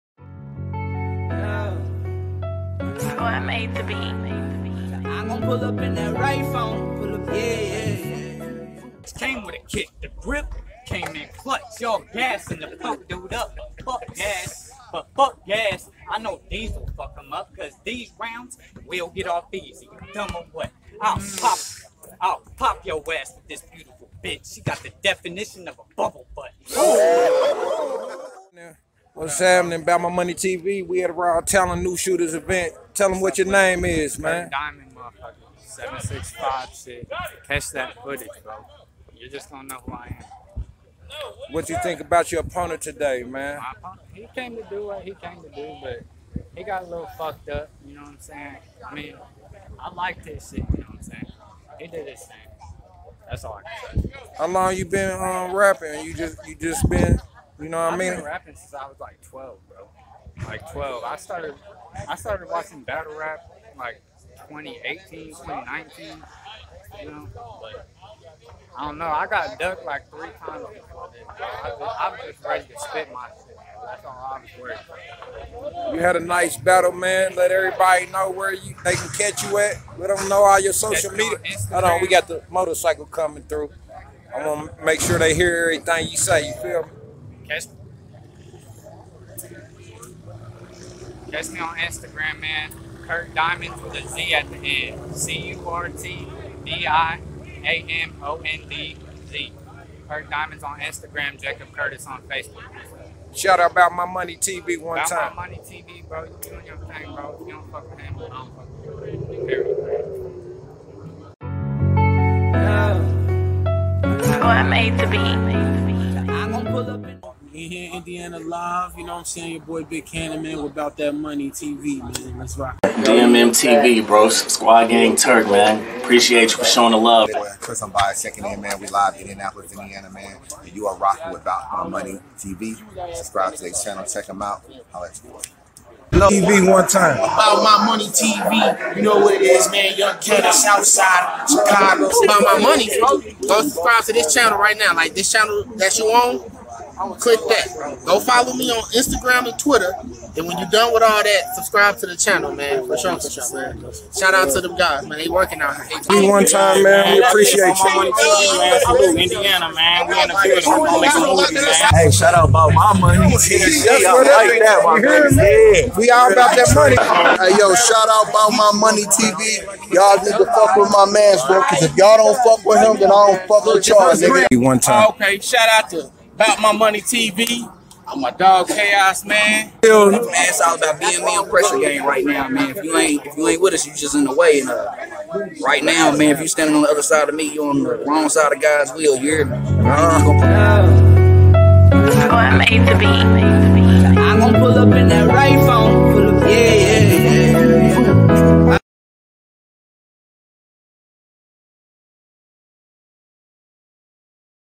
"New Shooter 🚨 Alert" Bout My Money TV 📺" Presents the New Shooters Spotlight 🔦 Curt Diamond
Bout My Money TV 📺" is starting a New Spitter Spotlight 🔦 we're looking like once a month bring you a new up and coming MC from the Midwest and throughout the U.S. If the League's keep up the 1SK's, Crucible's, and S1 cards then hopefully we can highlight someone new weekly to help raise the artist profiles this will be totally at random if I'm at a Event or if you the fans or MC's send us some dope footage then we will put in the work to get them on the Platform to help highlight their work.
Enough with the politics this Weeks New Spitter CurtDiamond reppin out of Indiana and RawTalentBattleLeague. BMMTv ran across Curt Diamond at RawTalent BattleLeague's New Shooters Event where he Battled another Dope new shooter Jim (who's interview is also up on the Channel) Curt Diamond was a surprisingly aggressive battler who had a decent flow with great wordplay. Be sure to check these two MCs on Raw Talent's Channel.

Also make sure you Follow us on all Platforms